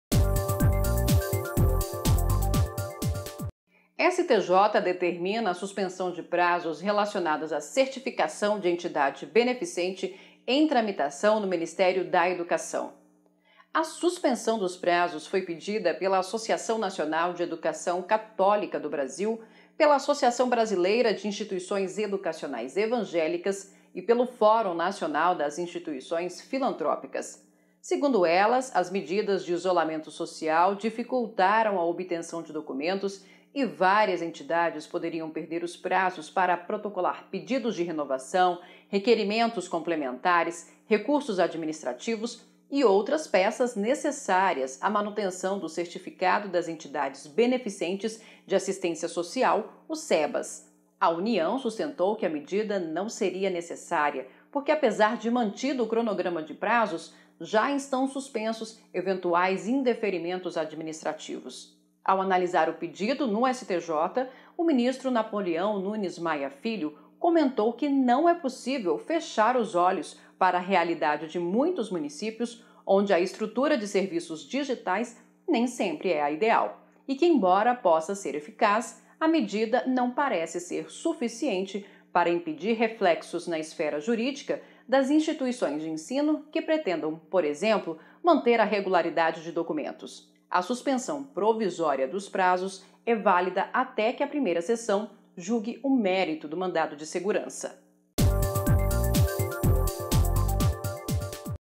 Determinada suspensão de prazos administrativos relacionados à certificação de entidade beneficente
​O ministro do Superior Tribunal de Justiça (STJ) Napoleão Nunes Maia Filho determinou a suspensão provisória dos prazos dos processos administrativos relativos à Certificação das Entidades Beneficentes de Assistência Social (Cebas) em tramitação no Ministério da Educação.